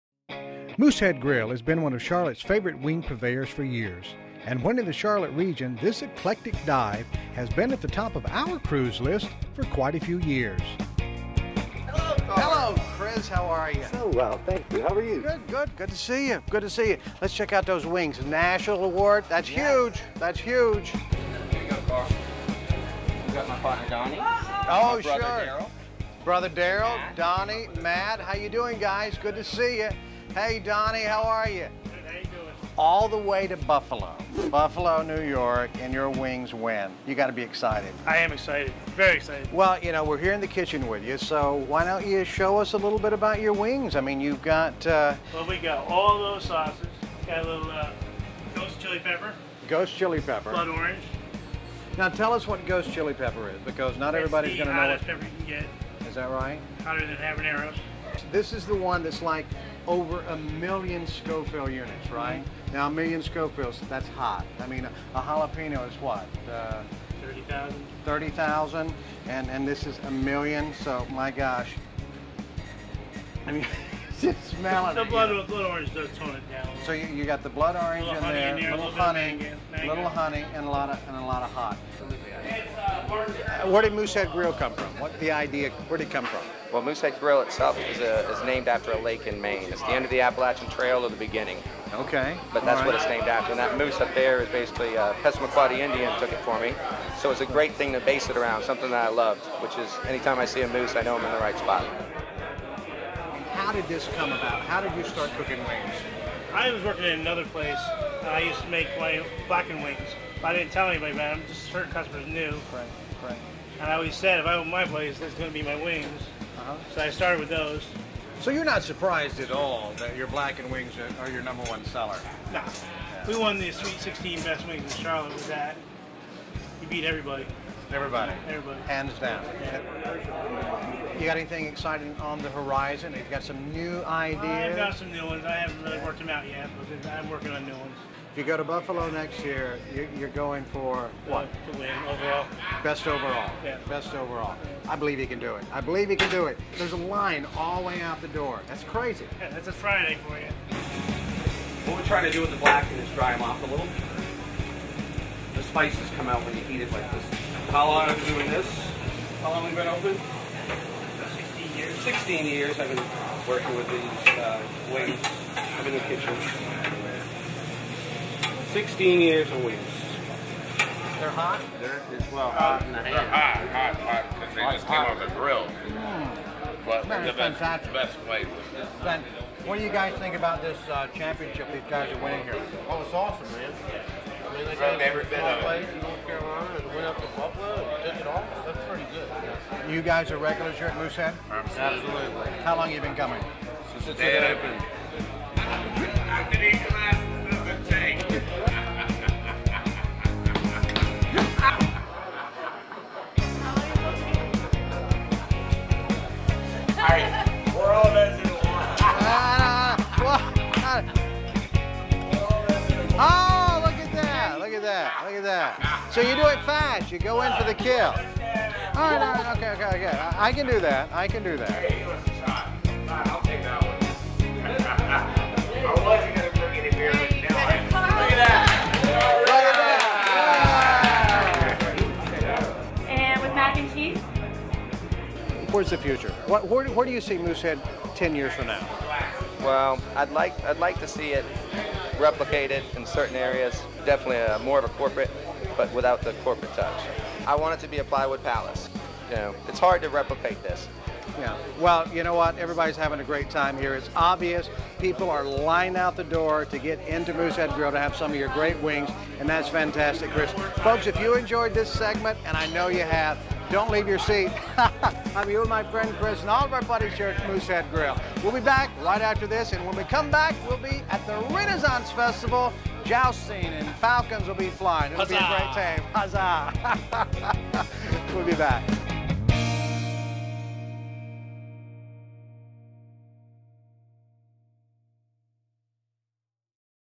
Moosehead Grill
Moosehead Grill in Charlotte has won national awards for their incredible BBQ wings.  Carl White gets behind the counter for a taste.